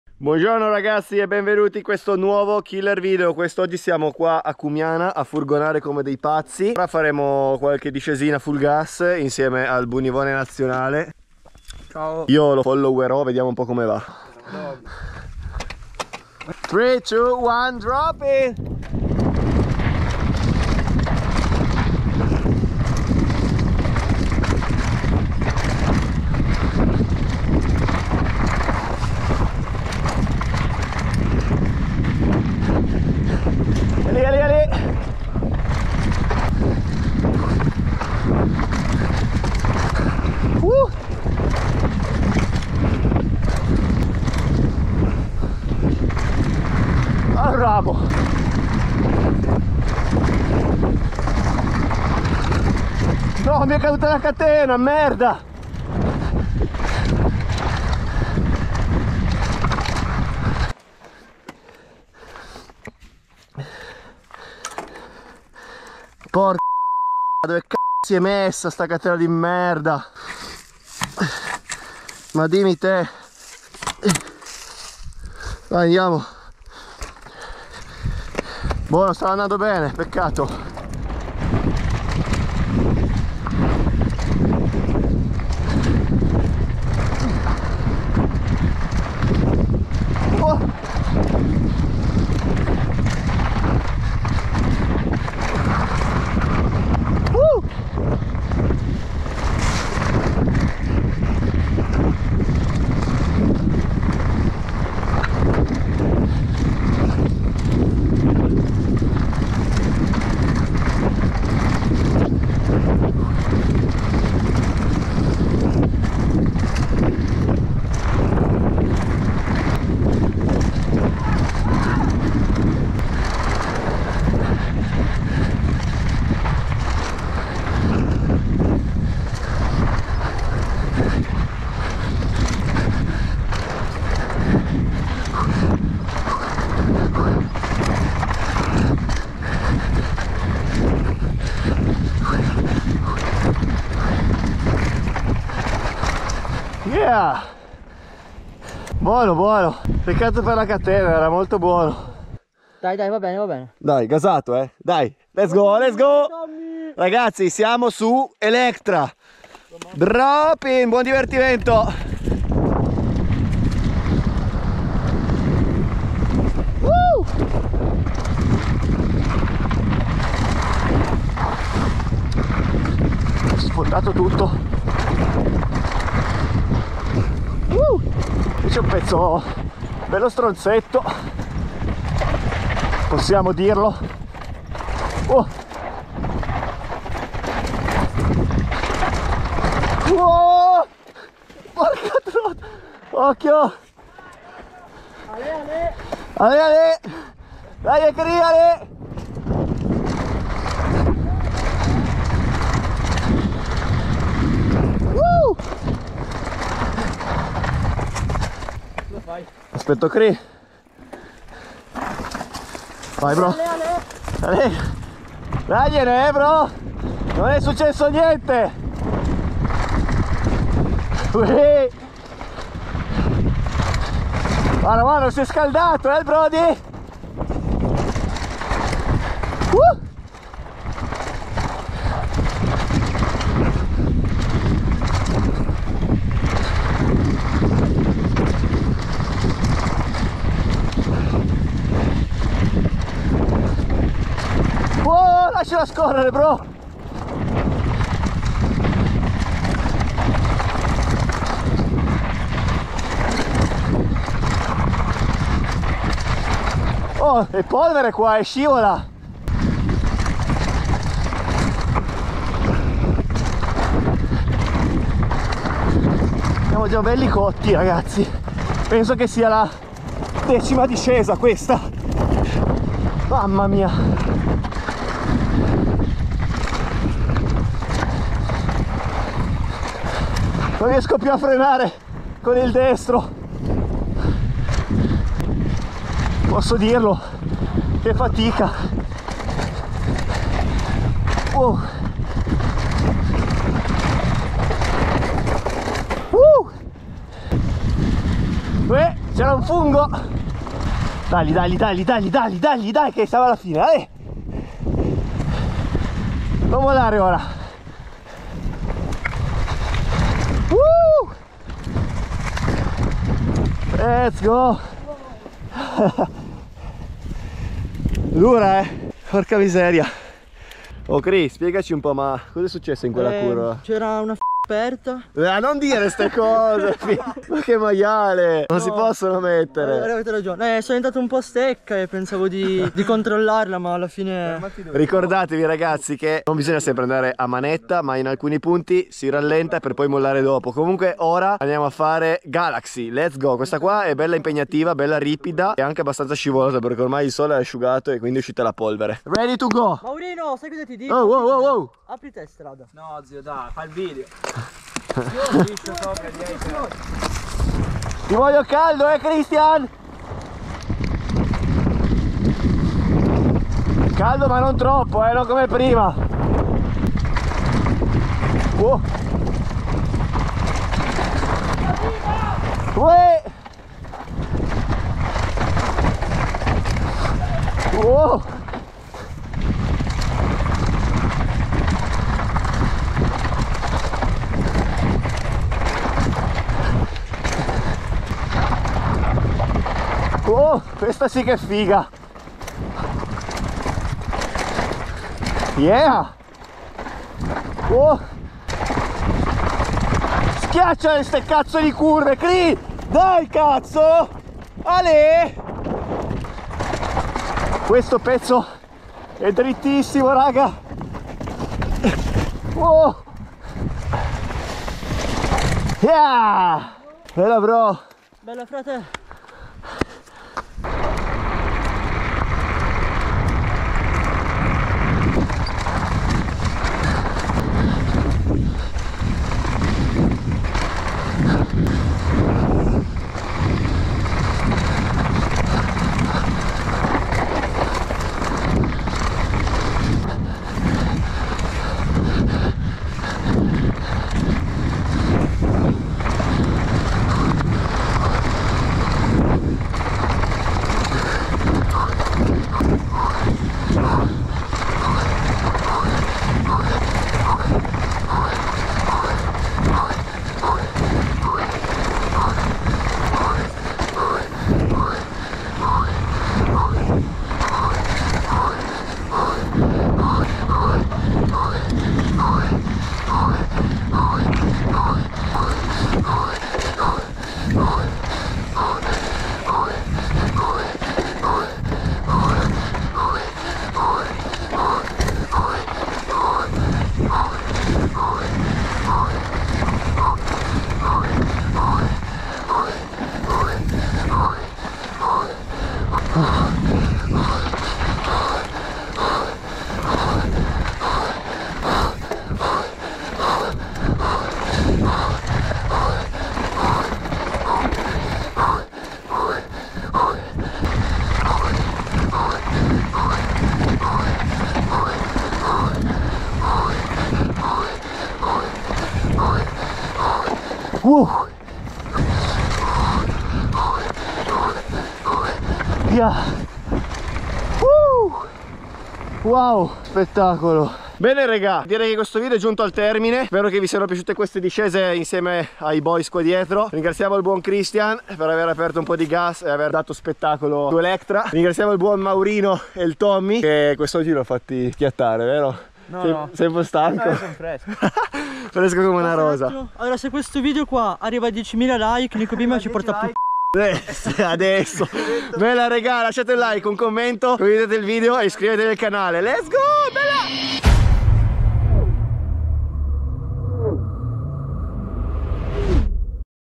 2 GIORNI DI FURGONATA EXTREMA con il TEAM VIGOR
Ciao ragazzi, benvenuti in un nuovo Killer Video. In questo video vi porto con me in 2 giorni di furganate pazze assieme ai ragazzi del team vigor. Il primo giorno abbiamo girato a Cumiana mentre il secondo nelle valli di Lanzo. Che trail spettacolari!! Dovete assolutamente venire a provaralii!

Buona visione!